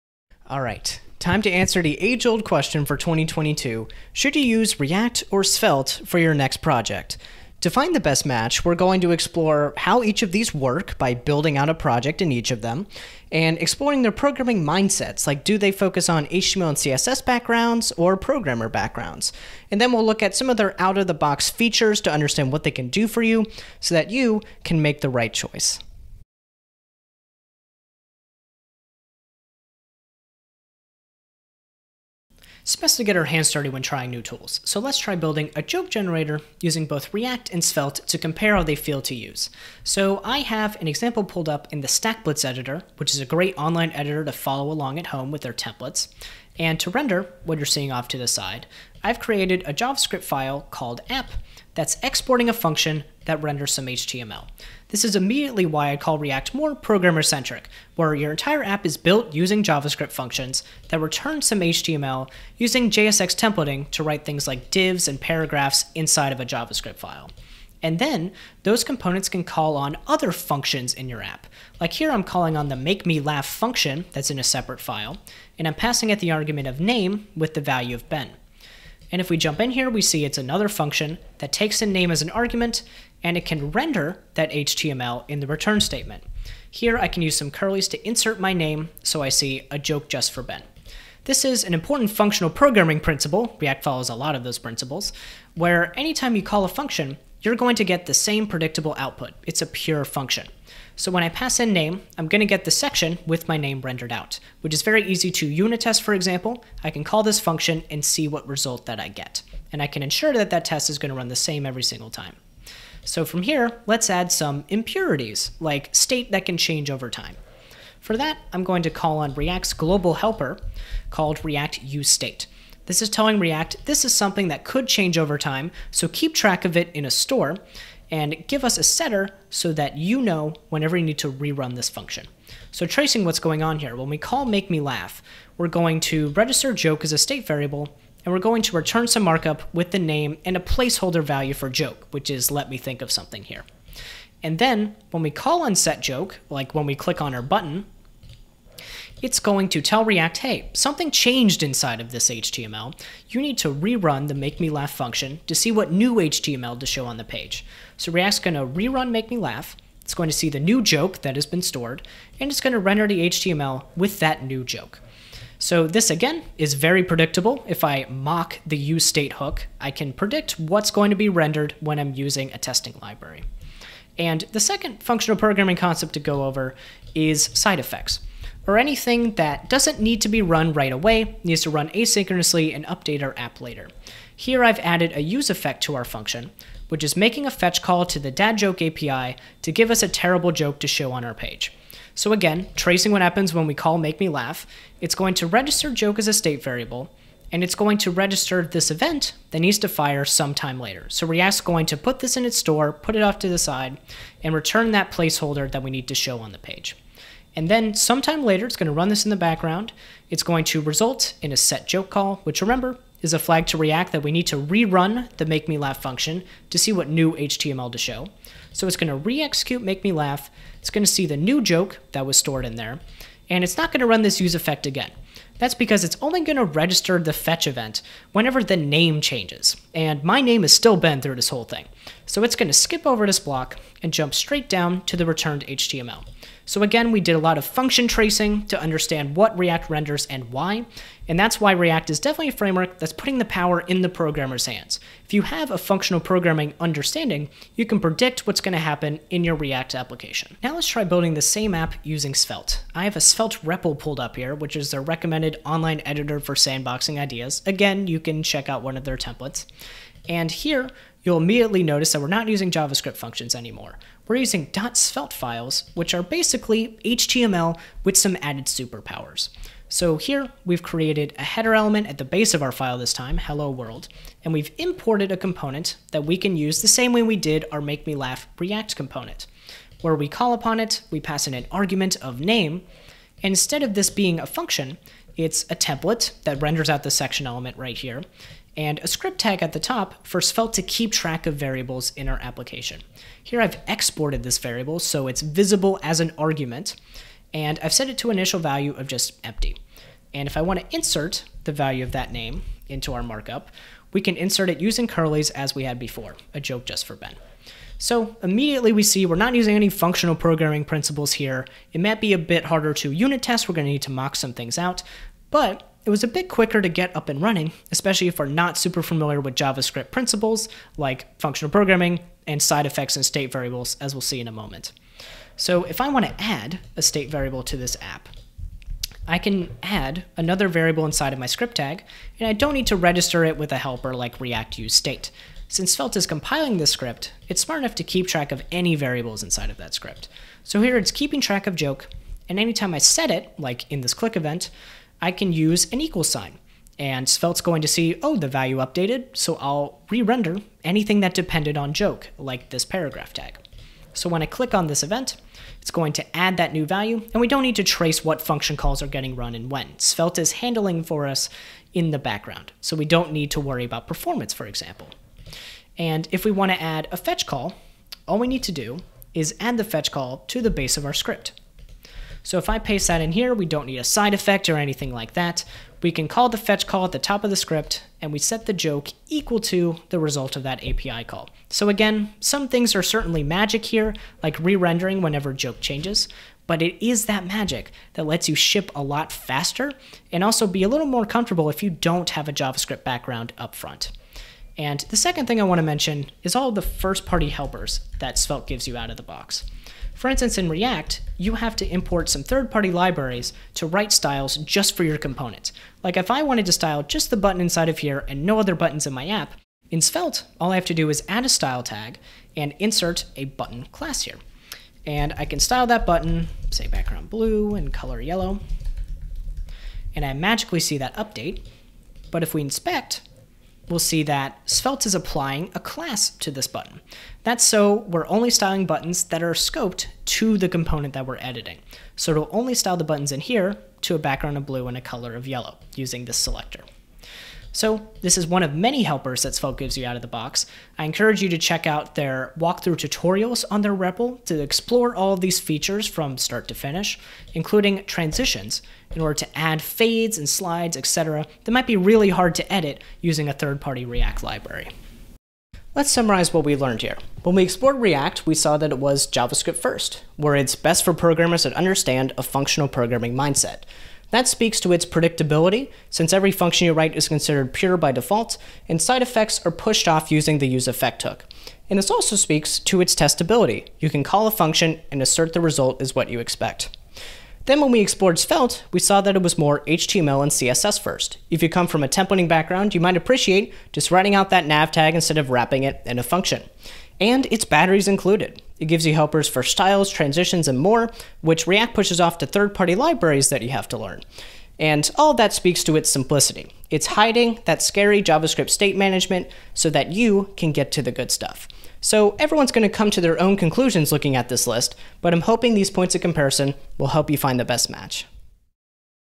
Svelte vs React in 2022: Choosing the Best Match for You
🎉 You can now build website with SvelteKit and Slice Machine, try it now for free:

When React is a good fit if Your team has a stronger programming background, your projects are extremely dynamic and data-driven and you plan to reuse your work across platforms.

When Svelte is a good fit if your team has a stronger front-end background, your projects are UI-centric and built for the web and your team needs to ship by yesterday.

Chapters:
📍 00:00 Intro
📍 00:26 Building with React - Jokes Generator
📍 04:11 Building with Svelte - Jokes Generator
📍 07:37 Summary Svelte vs React in 2022